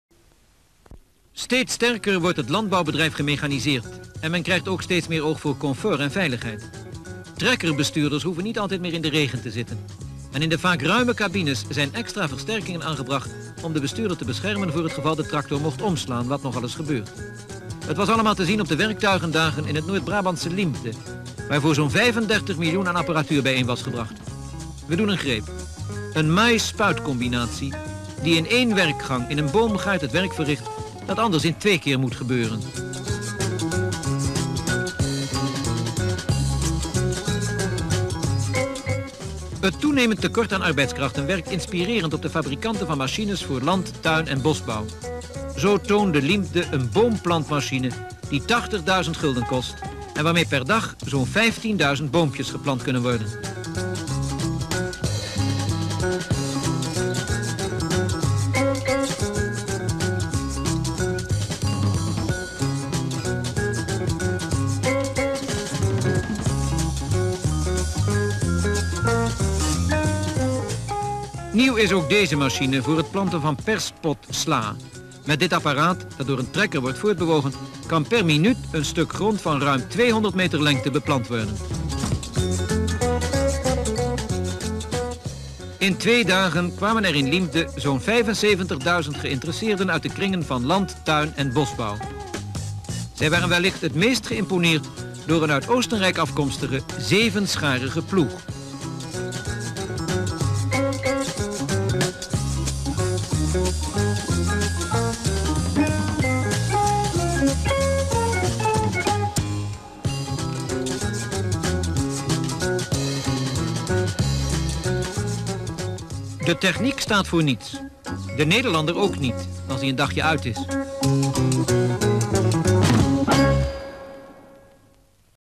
Agriculture-werktuigendag (Weeknummer 73-21) 🔴
Description (more): Agriculture-werktuigendag in Liempde, including tractors, a combine-syringe combination, machines for planting crops and trees and a zevenscharige team.